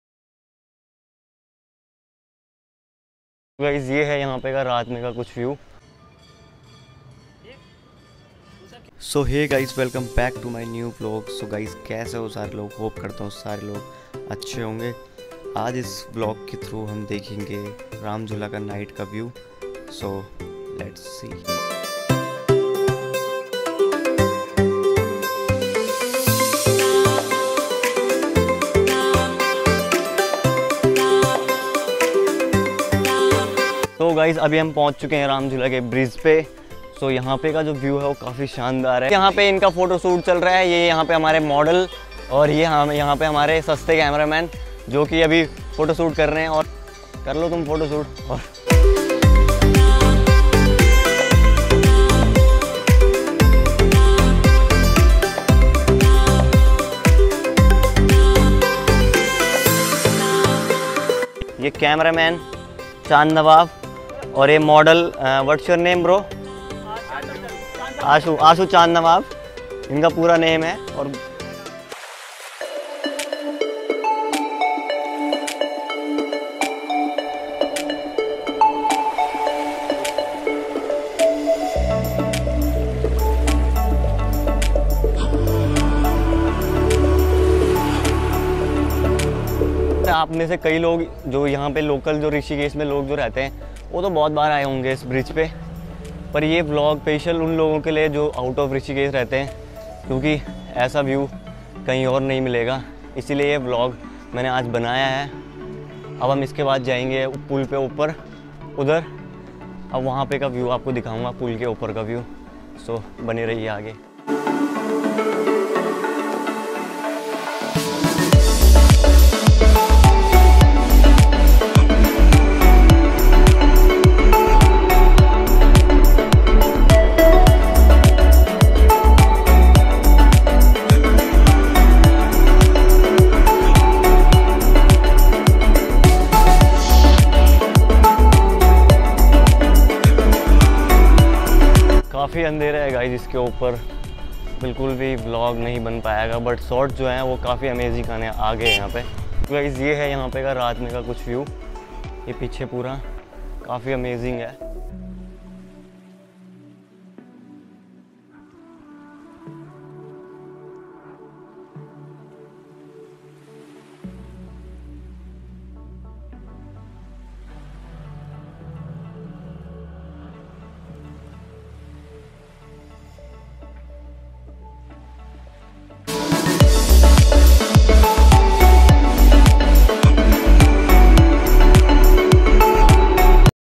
Beautiful evening in Ram jhula ||Rishikesh ||Saurav gairola Vlogs
This is the vlog of Ram jhula Rishikesh evening and night scene
This breeze is looking so dam in evening and night
I only suggest you you come in evening time because in evening there is looking so beautiful.
Music I used :no vlog copyright
I edit my vlog with the help of vn video editor
If you like the cinematic scenes